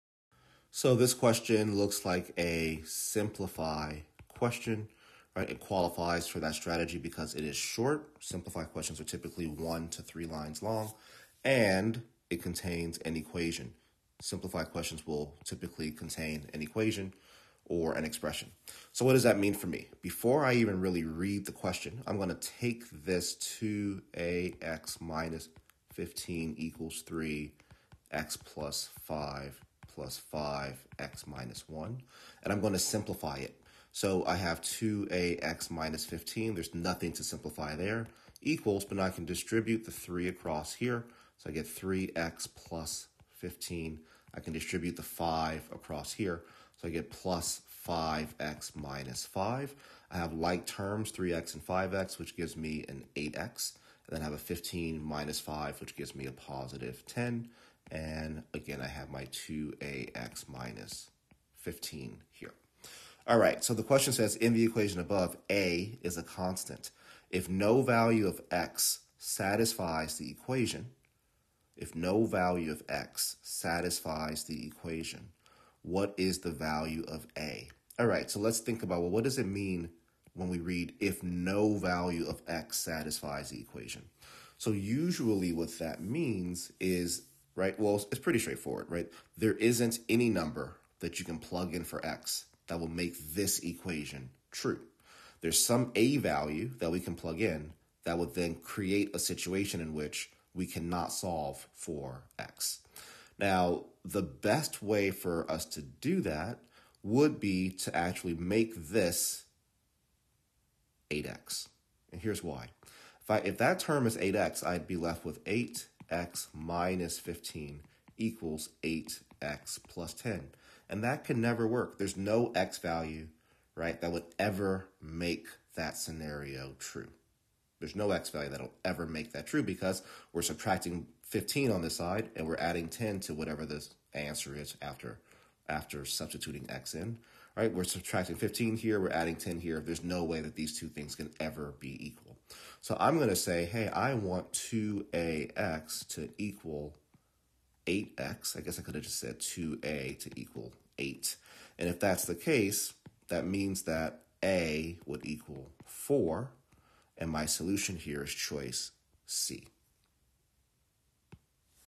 2ax - 15 = 3(x+5) +5(x-1). In the equation above, a is a constant. If no value of x satisfies....
Official SAT Practice Test 9, Section 3, Question 8:

2ax - 15 = 3(x+5) +5(x-1).  In the equation above, a is a constant.  If no value of x satisfies the equation, what is the value of a ?

For ACT math videos, check out @theactmathguy